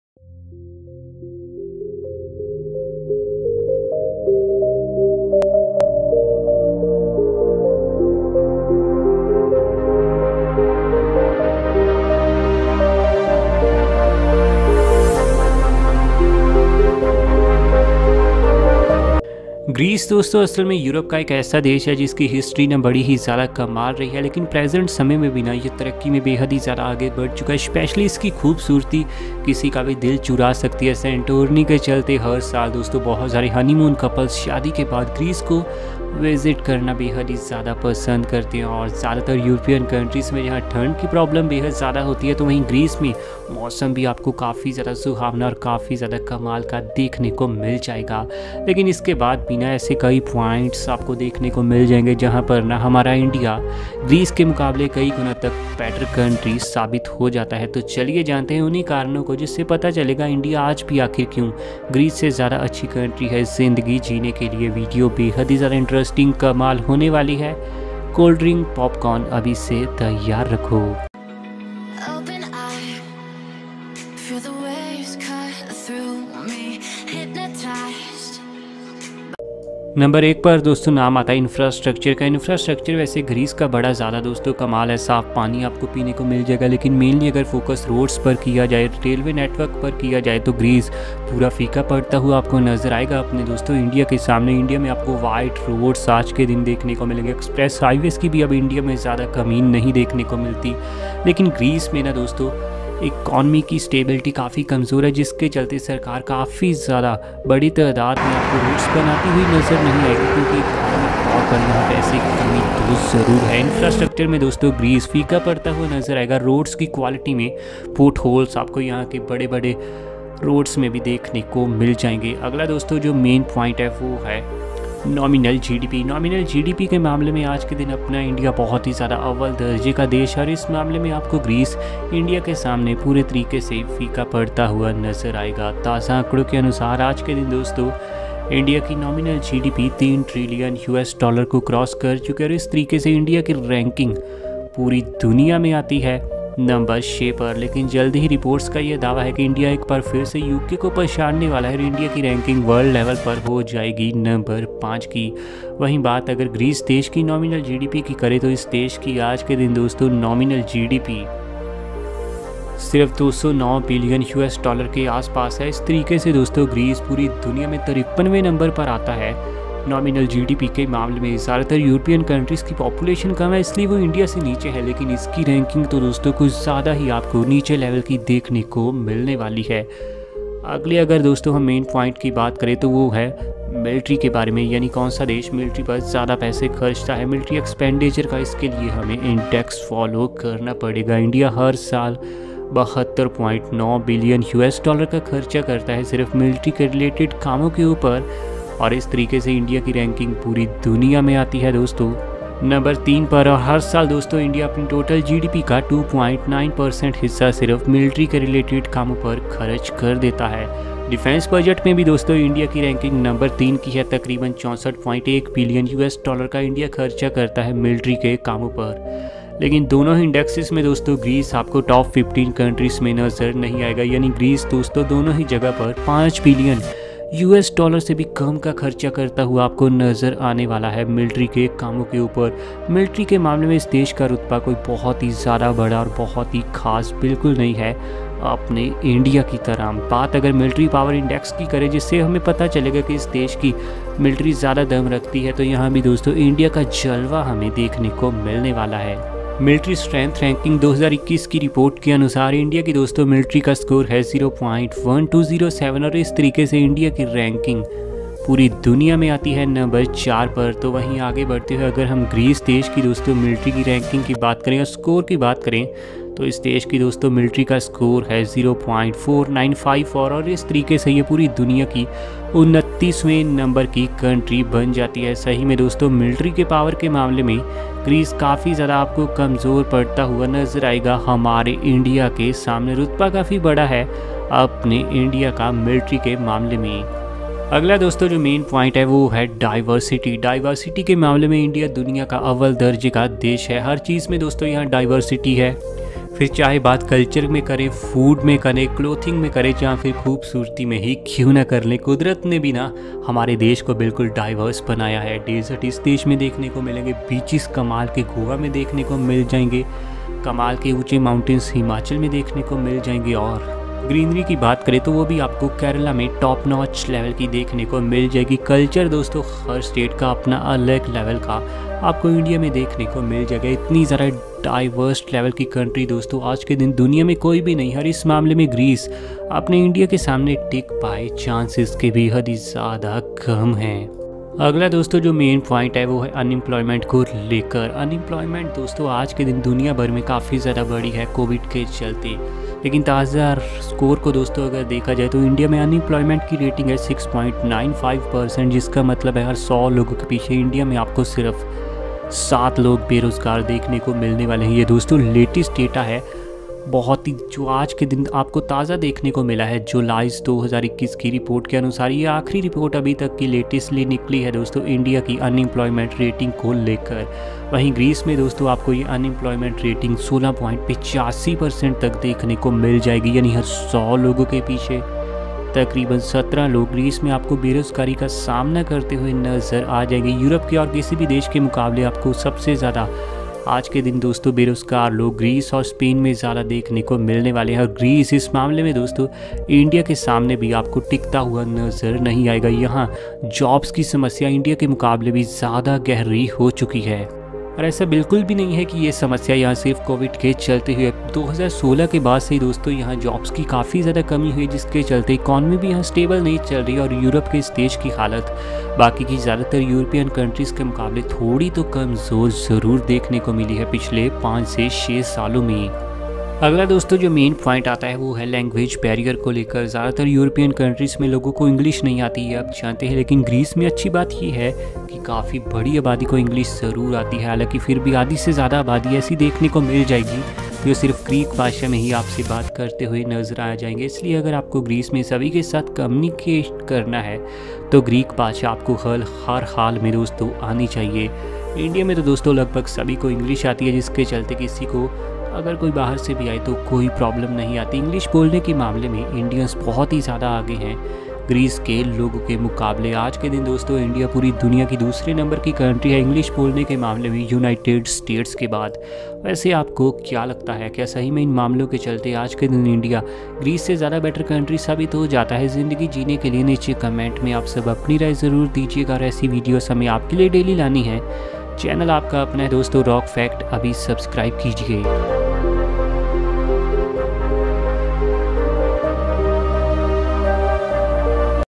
INDIA BETTER THAN GREECE || भारत ग्रीस से बेहतर देश है || REASONS INDIA BETTER THAN GREECE
HELLO  GUYS  HOW  R  U  ALL  ,  IN  THIS  VIDEO
  IM  TALK  ABOUT  WHY  INDIA  IS  BETTER  COUNTRY  THAN  GREECE.

DONT  FORGET  HIT :

L-I-K-E

S-H-A-R-E

S-U-B-S-C-R-I-B-E